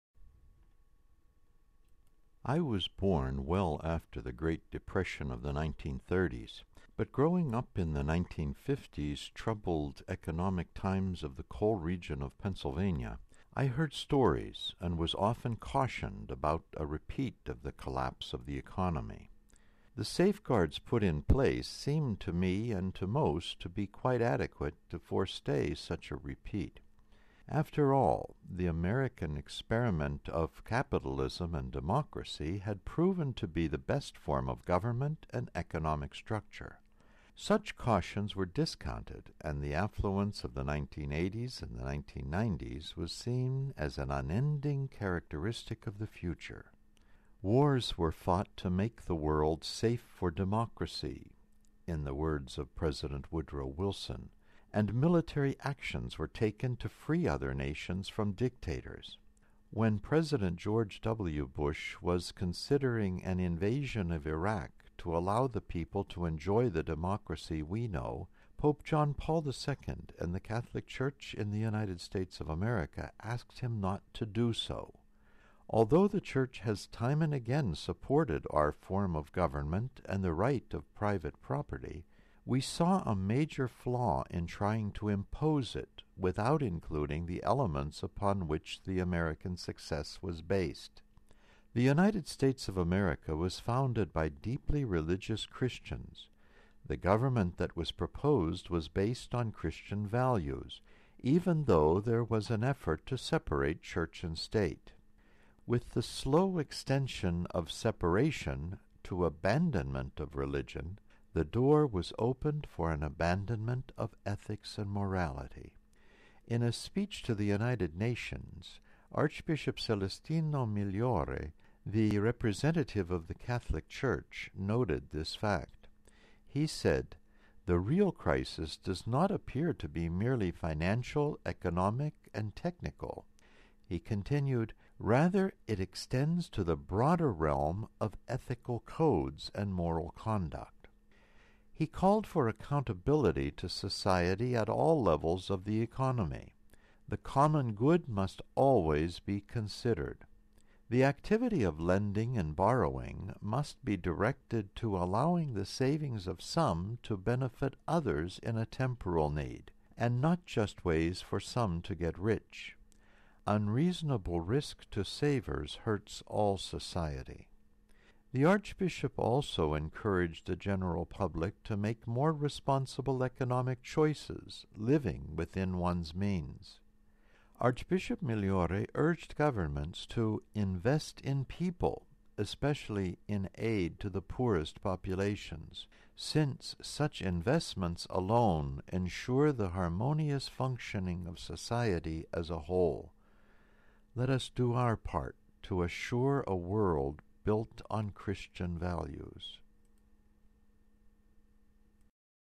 Real crisis about more than money
- Voice of the Shepherd - His Grace, The Most Rev. John Kudrick - Byzantine Catholic Eparchy of Parma, Ohio - November 9, 2008